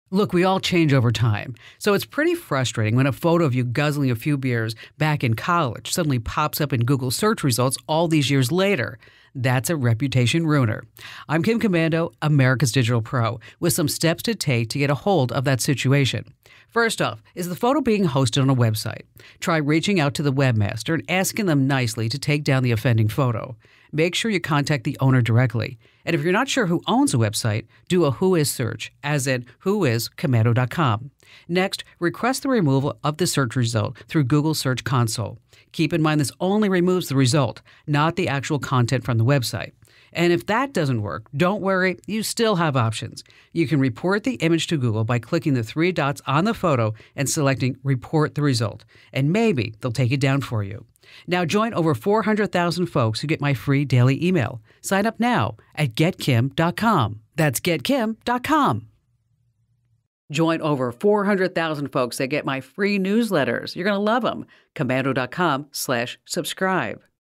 3 ways to remove your image from Google
Embarassing pics show up in Google's search engine? Here are the ways to take them down, in one minute.